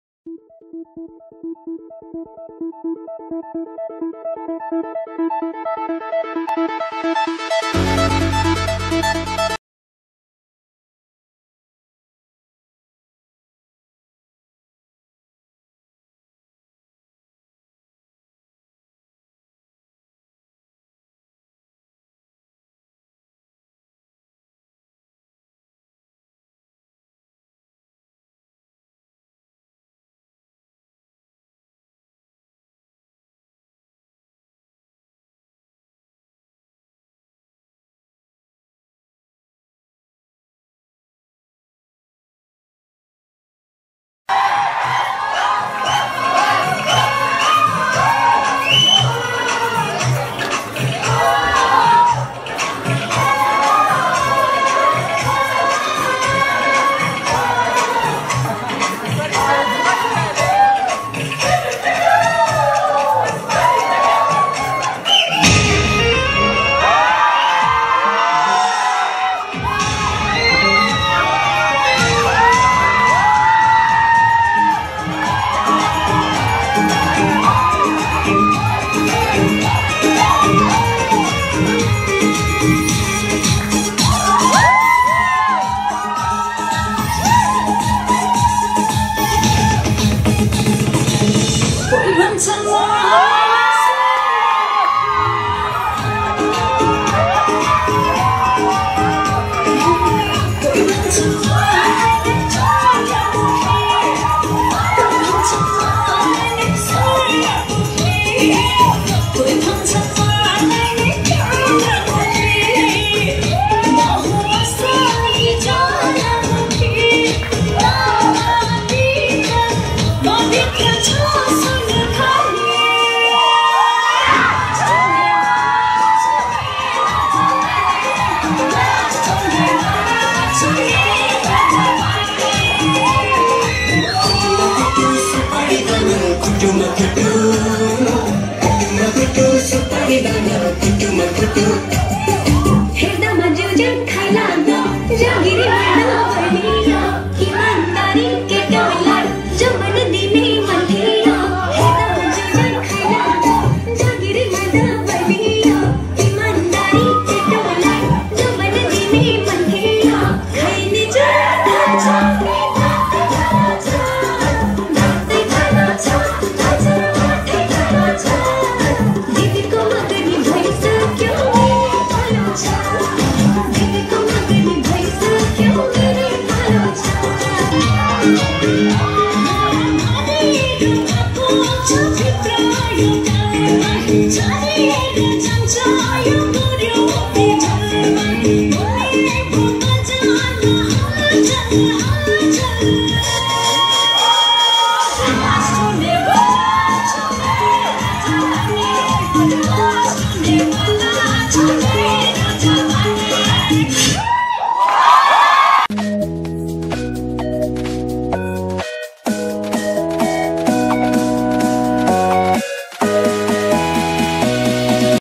Miss Anuka Chaudhary (Best Cultural present Winner) performance on Mr and Miss Tharu Talent show
Miss Anuka Chaudhary From itahari Sunsari won the subtitle * Best Cultural Present Award * on First Mr and Miss Tharu